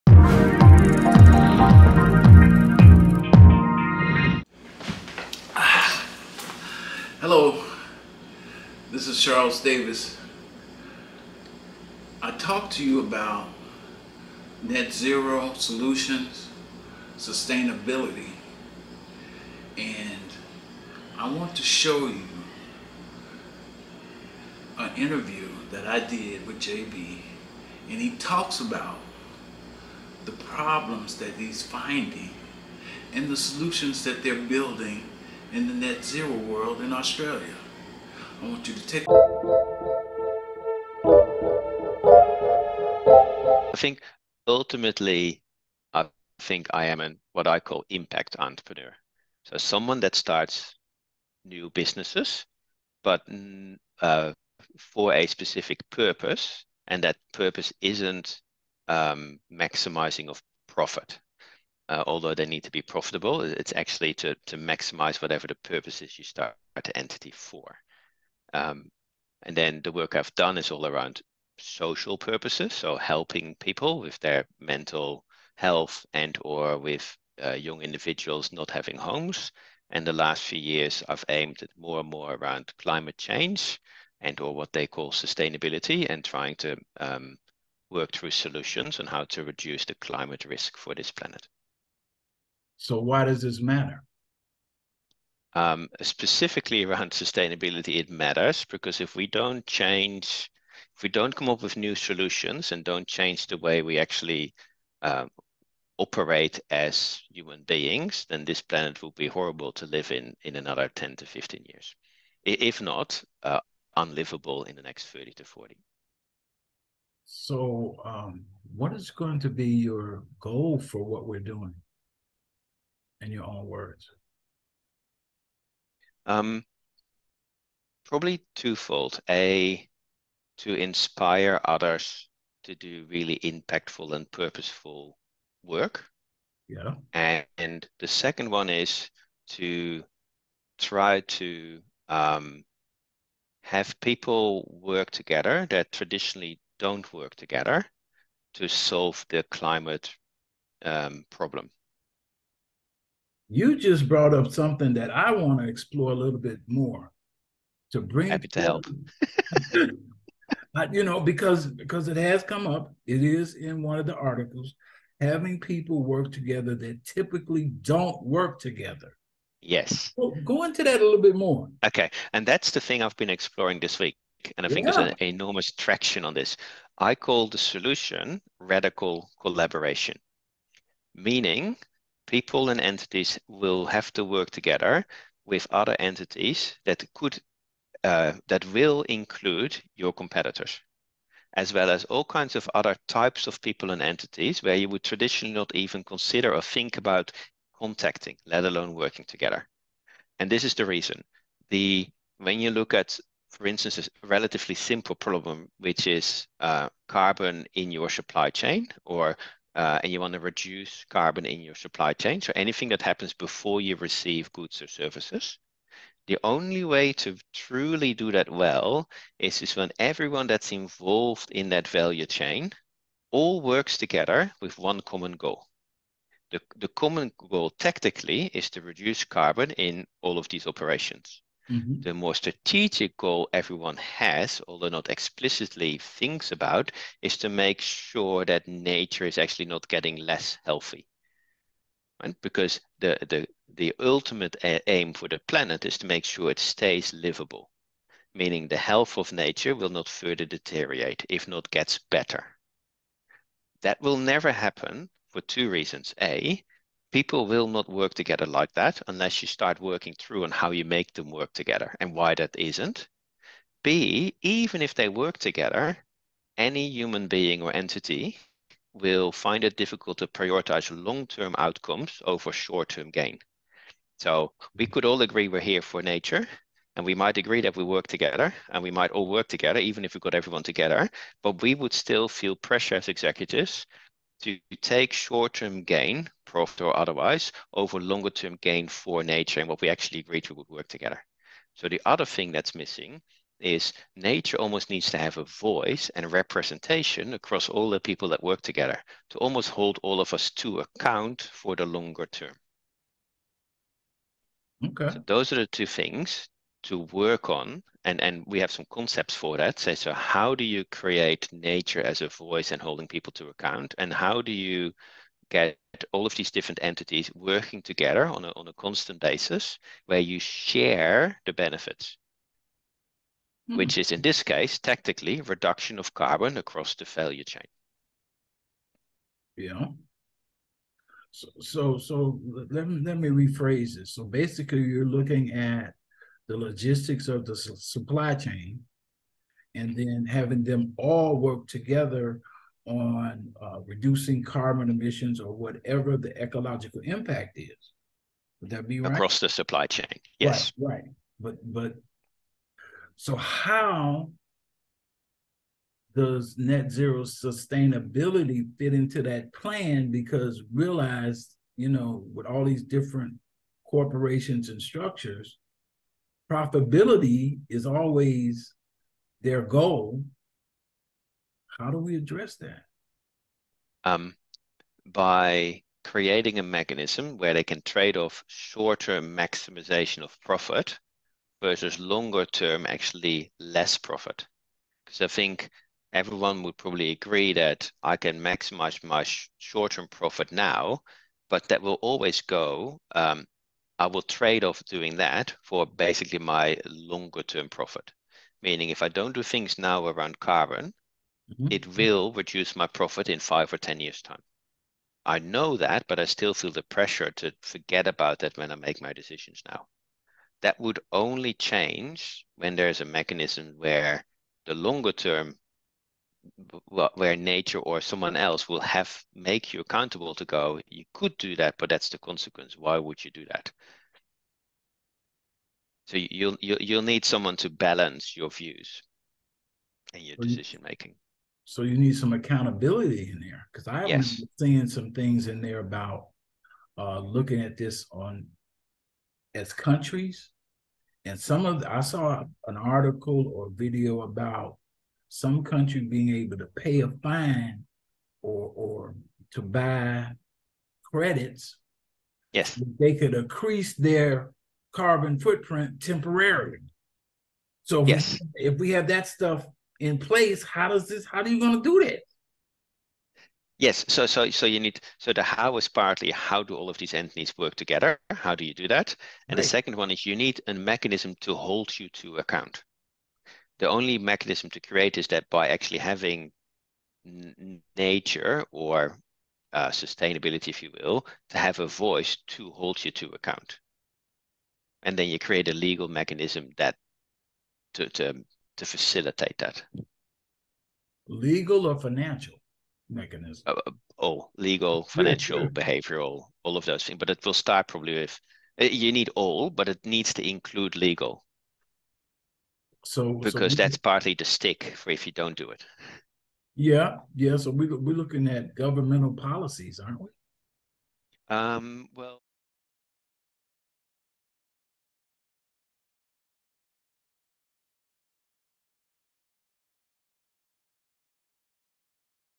Personal Brand Coaching Session with Jeroen Bolliujt from Australia
Are you looking for guidance to create a compelling brand strategy? Join Jeroen Bolliujt as he shares his mission of creating a personal brand that stands for ecology. Learn how to build engaging video content and culture-building that will inspire your customers and followers, and position your company as an industry leader. See how we coach personal brands into success by helping them refine their message. Tune in now to get the insights you need for success!